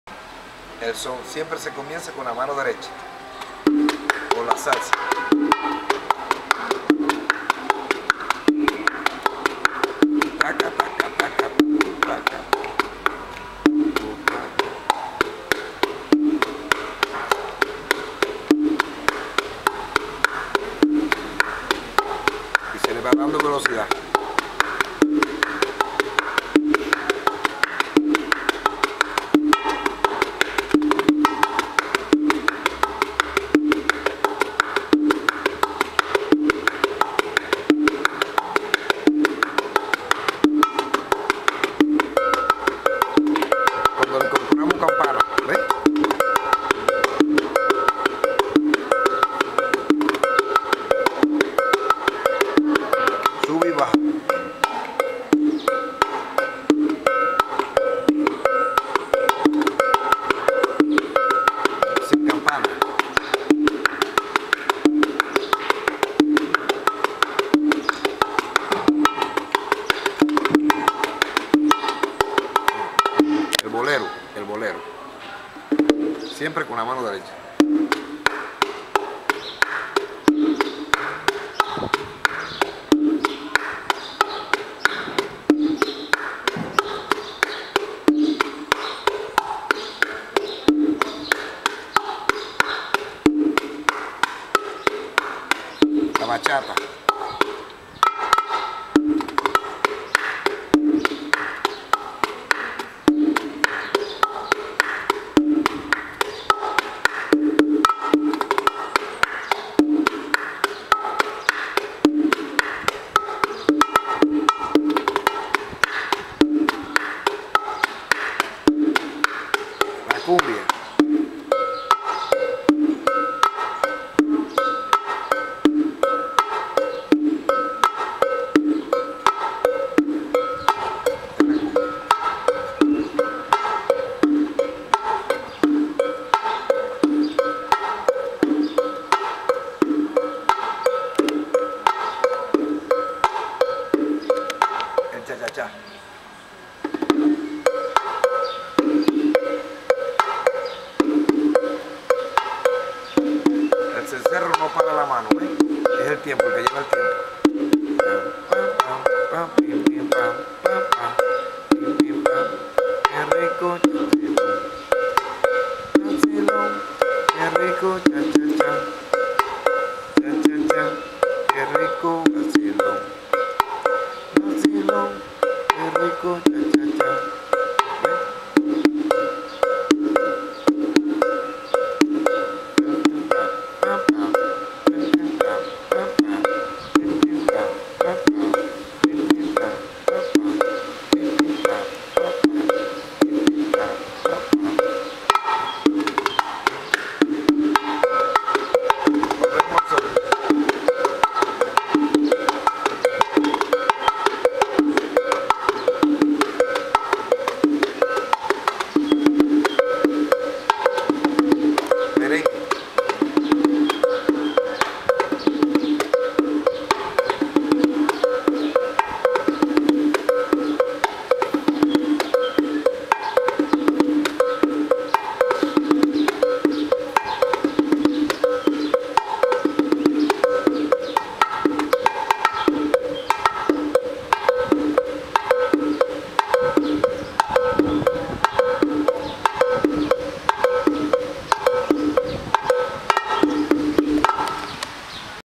Osiris Lorenzo Pena - Cuban rhythms on bongos
Playa Esmeralda, Hotel Sol Rio de Luna y Mares, 2018.
Lessons on bongos...